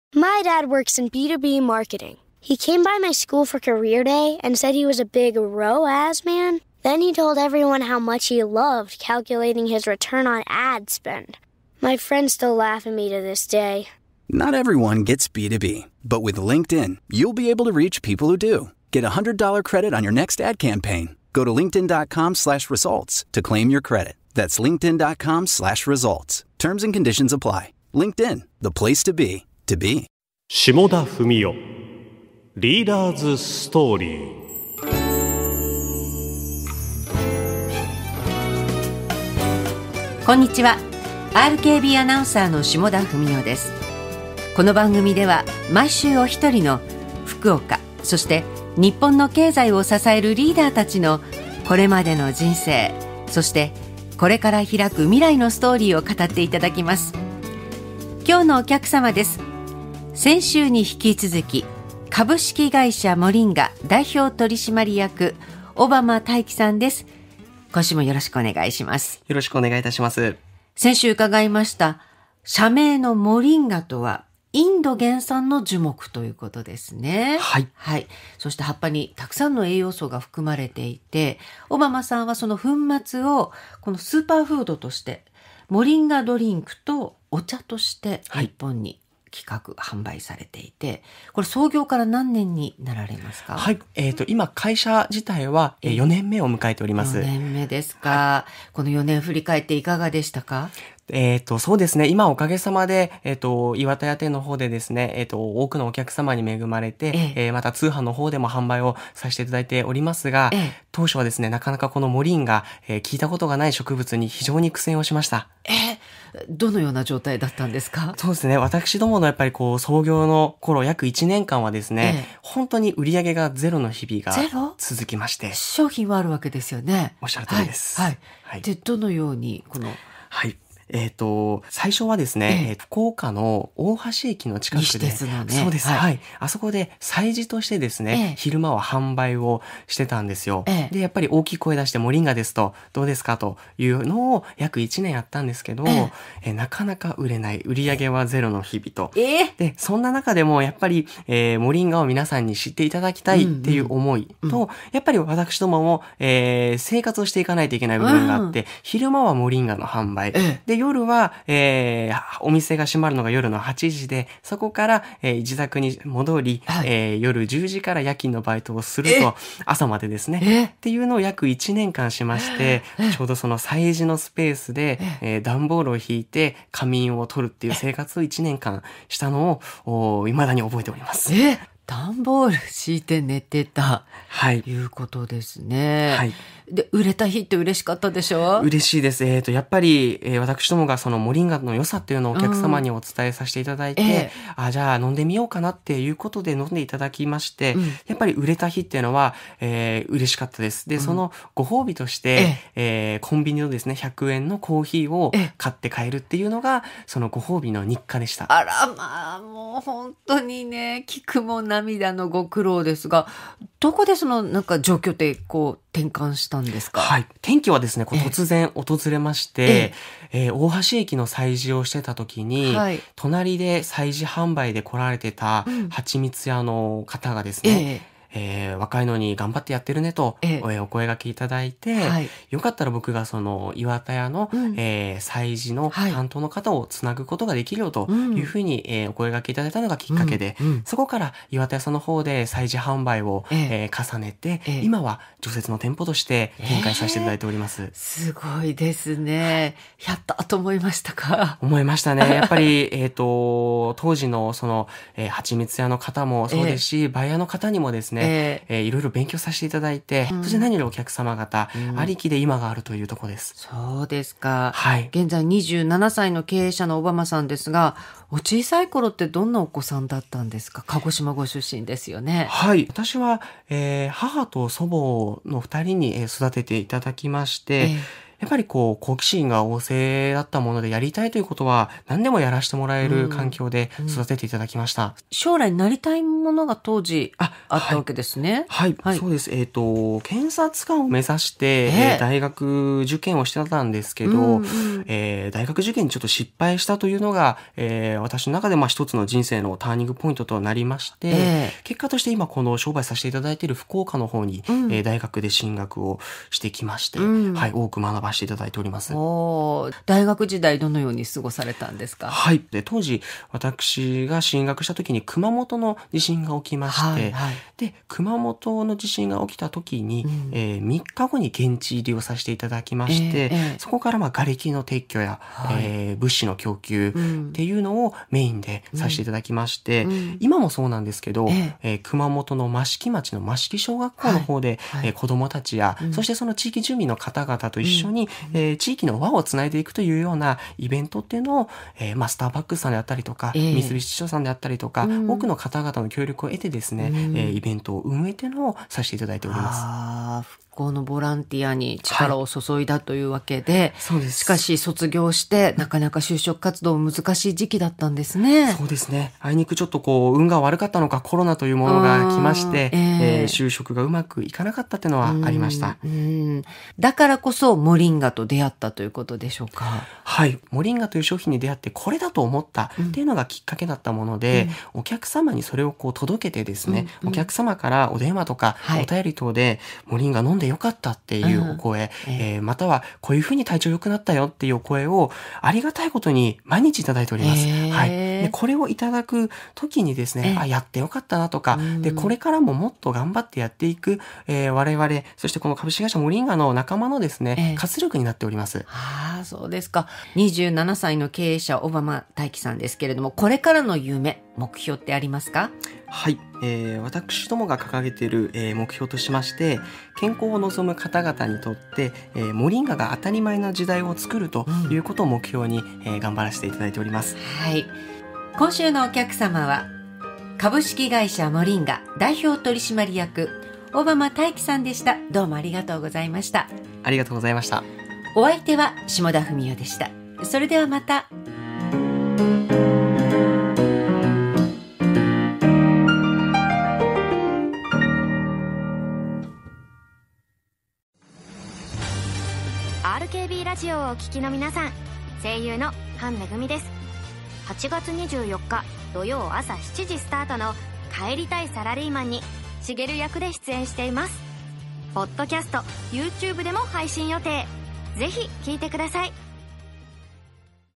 2024年8月12日放送 株式会社モリンガ 代表取締役 小濱大貴さん
2024年8月12日放送

株式会社モリンガ

代表取締役　小濱大貴さん

「モリンガ」とは栄養が豊富な植物の名前、主に赤道直下の暖かい地域に自生しています。葉の部分を青汁のようにして摂取することにより様々な効果が得られる健康食品として販売しています。

代表の小濱さんは大学を出た時が新型コロナの時期と重なり就職に失敗。その時実家の庭に植える木を捜していた時に「モリンガ」に出会いました。その不思議な名前に興味を持ち色々と調べてみてその効能に驚き、商品化を決意、会社を設立しました。

駅の催事場による販売からスタートしましたが最初はほとんど売れず、苦労を重ねましたがその時の出会いにより岩田屋での催事販売が出来るようになりました。その結果売り上げも評判も上々、現在では岩田屋に店舗を持ち通信販売も始めました。

放送記録

2024年8月12日